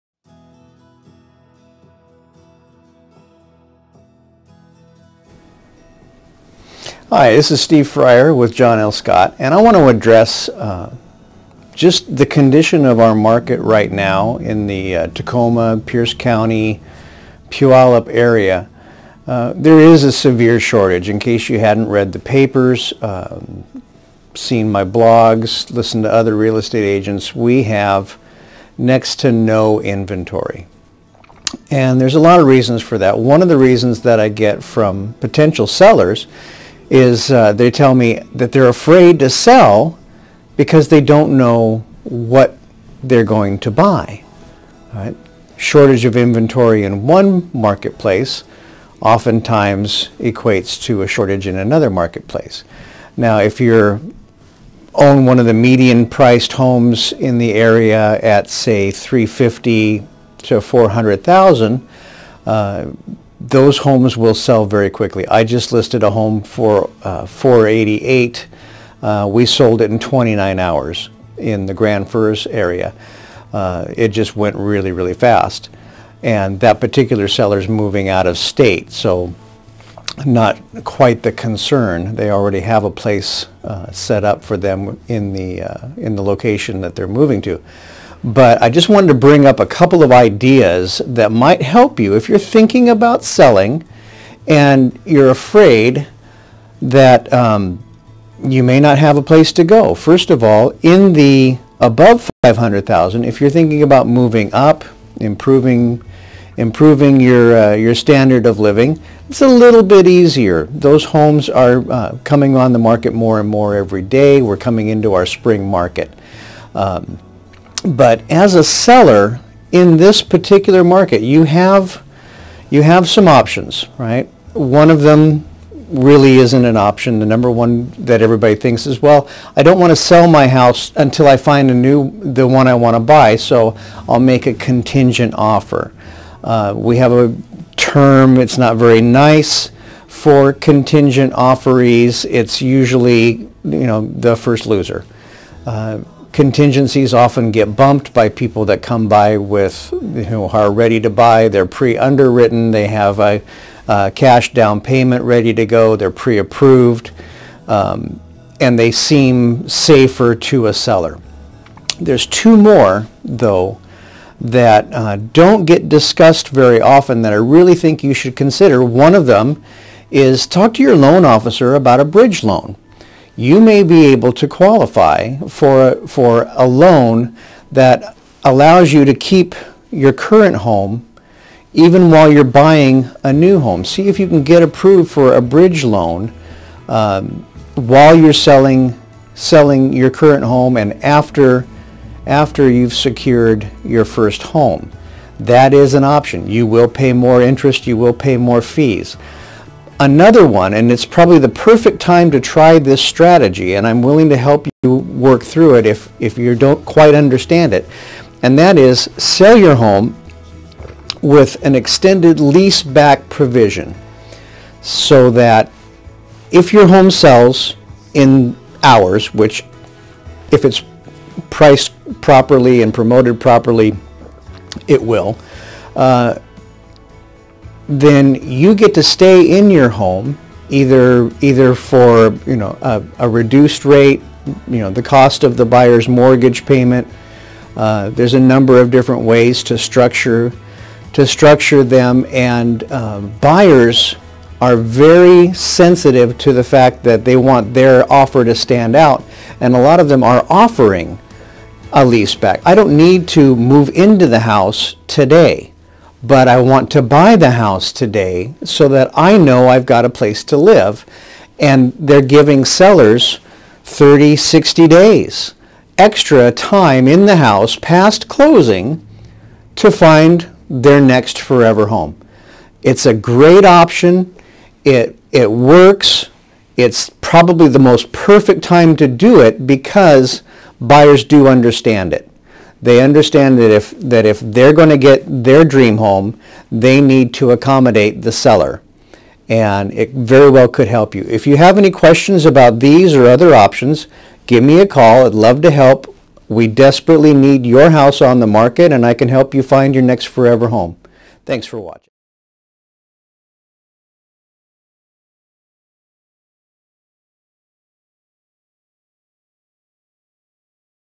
Selling Your Home When There's "Nothing" To Buy
Strategies to help you transition from the house you're selling to the one you want to buy.